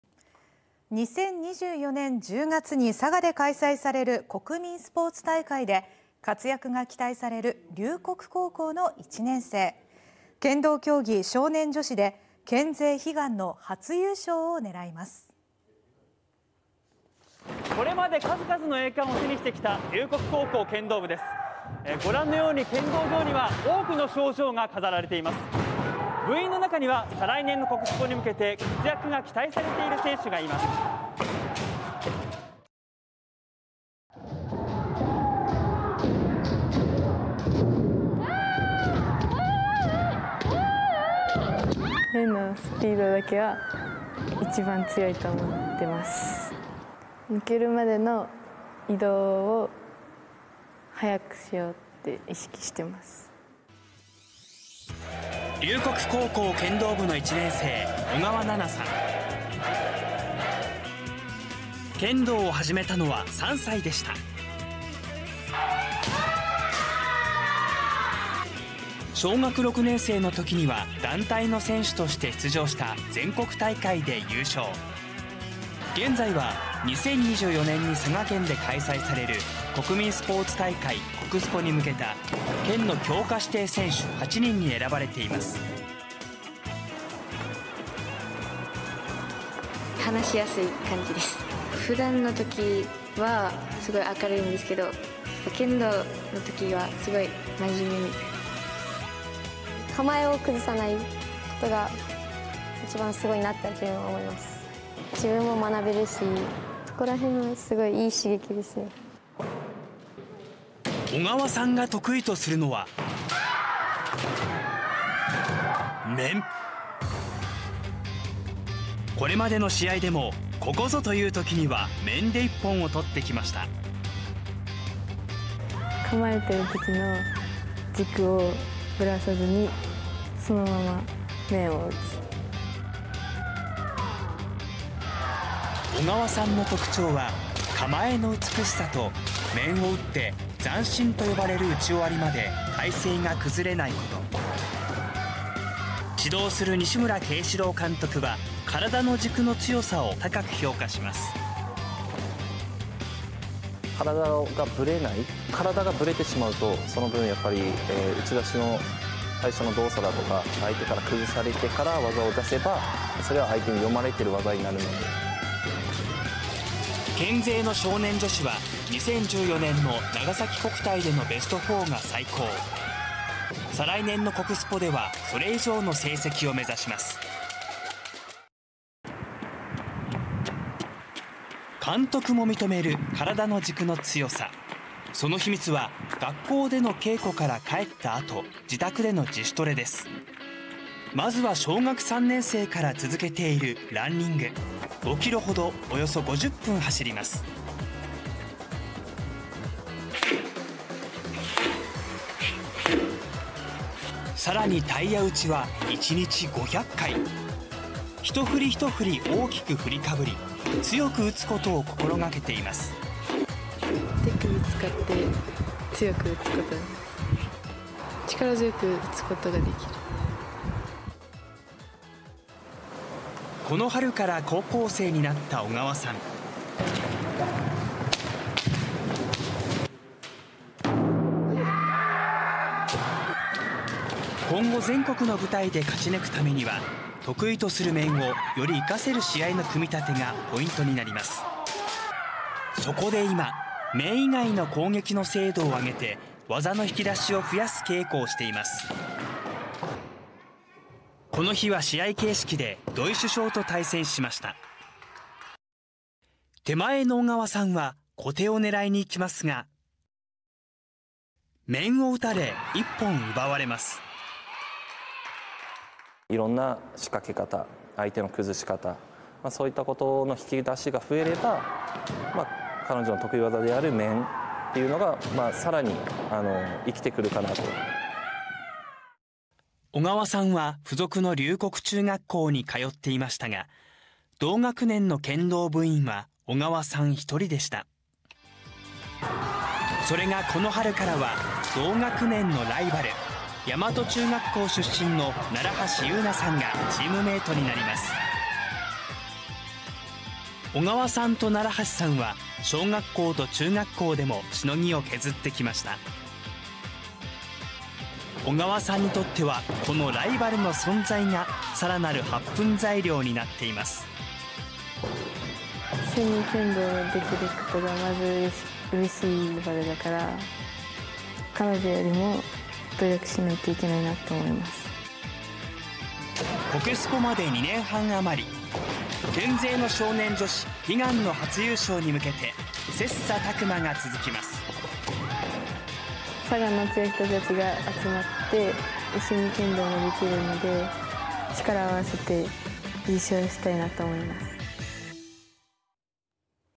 国スポで県勢悲願の初優勝を狙う 龍谷高校の剣道女子【佐賀県】 (22/04/06 18:30)
2024年10月に佐賀で開催される国民スポーツ大会で活躍が期待される龍谷高校の1年生を紹介します。剣道競技、少年女子で県勢悲願の初優勝を狙います。

【リポート・波佐間崇晃】
「数々の栄冠を手にしてきた龍谷高校剣道部です。剣道場には多くの賞状が飾られています。部員の中に国スポに向けて活躍が期待されている選手がいます」

【龍谷高校1年 小川ななさん】
「面のスピードだけは、一番強いと思っています。抜けるまでの移動を速くしようと意識しています」

龍谷高校剣道部の1年生、小川ななさん（神埼市）。
剣道を始めたのは3歳でした。小学6年生の時には団体の選手として出場した全国大会で優勝。現在は2024年に佐賀県で開催される国民スポーツ大会、国スポに向けた県の強化指定選手8人に選ばれています。

【龍谷高校3年 新田七乃葉さん】
「小川さんは話しやすい感じです。普段はすごい明るいんですけど、剣道の時はすごい真面目」

【龍谷高校3年 土井清佳主将】
「構えを崩さないことが一番すごいなという風に思います。自分も学べるし、そこらへんはすごい良い刺激です」

小川さんが得意とするのは「面」。
これまでの試合でも、ここぞいうときには、「面」で一本を取ってきました。

【小川さん】
「構えている時の体の軸をぶらさずに、ぶれないようにそのまま面を打つ」

小川さんの特長は、構えの美しさと、面を打って、残心（ざんしん）と呼ばれる打ち終わりまで体勢が崩れないこと。
指導する西村慶士郎監督は、「体の軸の強さ」を高く評価します。

【龍谷高校 西村慶士郎監督】
「小川さんは体がぶれない。体がぶれてしまうと、その分打ち出しの最初の動作だとか、相手から崩されてから技を出せば、それは相手に読まれてる技になる」

県勢の少年女子は、2014年の長崎国体でのベスト4が最高。2024年の国スポでは、それ以上の成績を目指します。

監督も認める体の軸の強さ。その秘密は、学校での稽古から帰ったあと、自宅での自主トレです。まずは小学3年生から続けているランニング。5キロほど、約50分走ります。
さらにタイヤ打ちは1日500回。一振り一振り、大きく振りかぶり、強く打つことを心掛けています。

【小川さん】
「意識しているのは、手首を使って強く打つことです。力強く打つことができる」

この春から高校生になった小川さん。今後、全国の舞台で勝ち抜くためには、得意とする「面」を、より生かせる試合の組み立てがポイントです。そこで今、面以外の攻撃の精度を上げて、技の引き出しを増やす稽古をしています。

この日は試合形式で土井主将と対戦しました。手前の小川さんは小手を狙いに行きますが…。面を打たれ1本奪われます。

【西村監督】
「いろんな仕掛け方、相手の崩し方、そういった引き出しが増えれば、彼女の得意技である面が更に生きてくる」

小川さんは附属の龍谷中学校に通っていましたが、同学年の剣道部員は小川さん1人でした。それがこの春からは同学年のライバル、大和中学校出身の楢橋優那さんがチームメイトになります。
小川さんと楢橋さんは小学校と中学校でもしのぎを削ってきました。小川さんにとってはこのライバルの存在が更なる発奮材料になっています。

「楢橋さんと一緒に剣道できることがまずうれしい。ライバルだから彼女よりも努力しないといけないと思います」

国スポまで2年半あまり。県勢の少年女子、悲願の初優勝に向けて、切磋琢磨が続きます。

「国スポでは佐賀の強い人たちが集まって一緒に剣道ができるので、力を合わせて優勝したいなと思います」